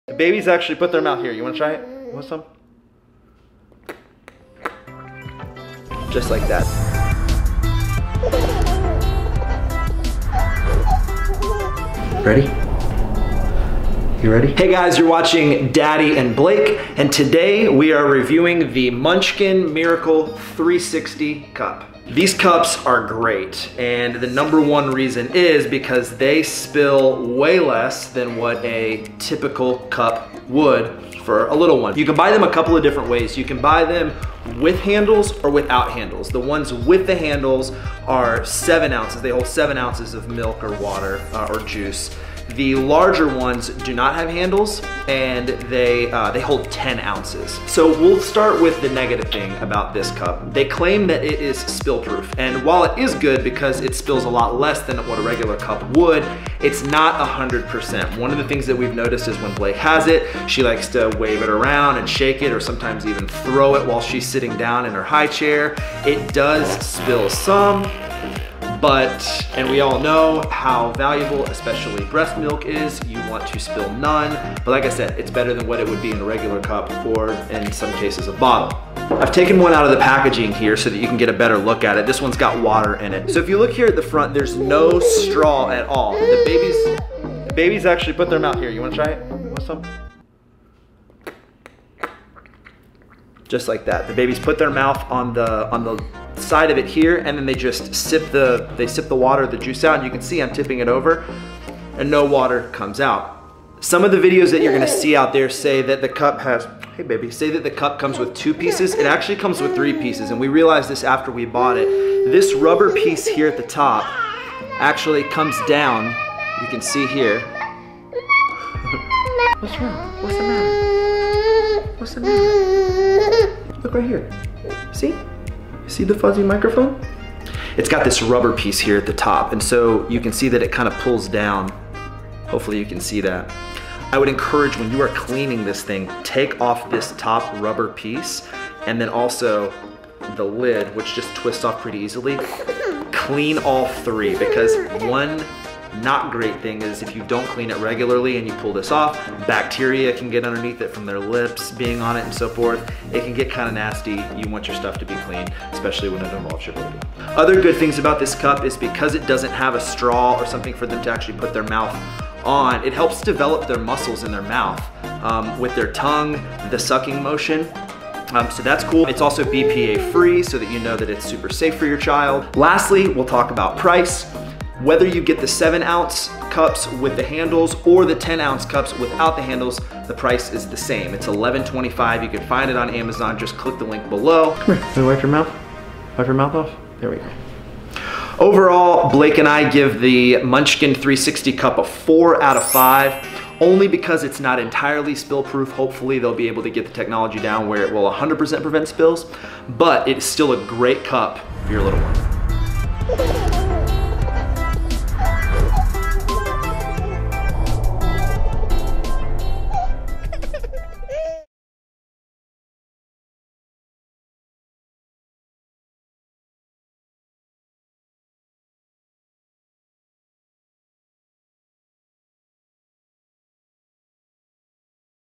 Munchin 360 Miracle cup REVIEW
The Miracle 360 cup is pretty miraculous. It helps prevent spills and is easy for infants to handle and teach themselves how to drink from a cup.

You can purchase the cup here:

WITH HANDLES
WITHOUT HANDLES

MUSIC